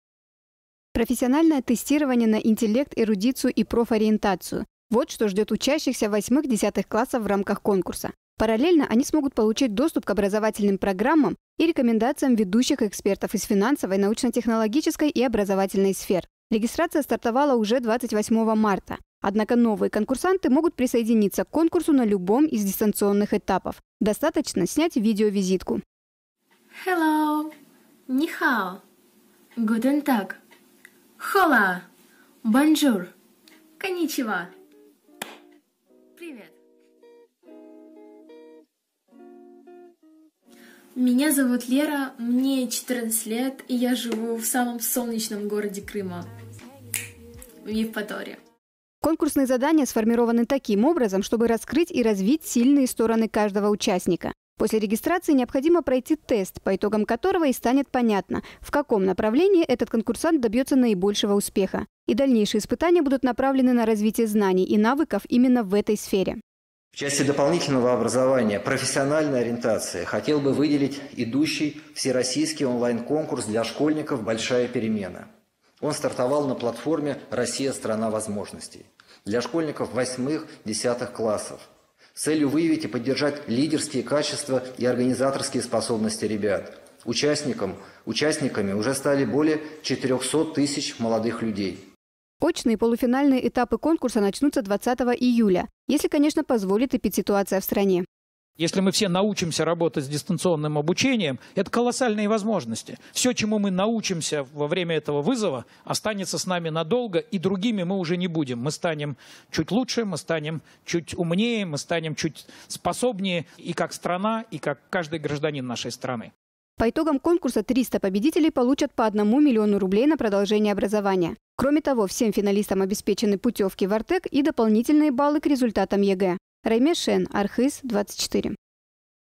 Школьники из КЧР включились в федеральный конкурс «Большая перемена»
Большие перемены в последнее время коснулись абсолютно всех. И система образования одной из первых перестроилась под новые реалии. Еще в марте в России стартовал конкурс для старшеклассников «Большая перемена», организованный на платформе «Россия — страна возможностей». Конкурс примечателен тем, что в нем нет правильных ответов и готовых решений. Чтобы стать победителем нужны свежие идеи и неординарный взгляд на вещи.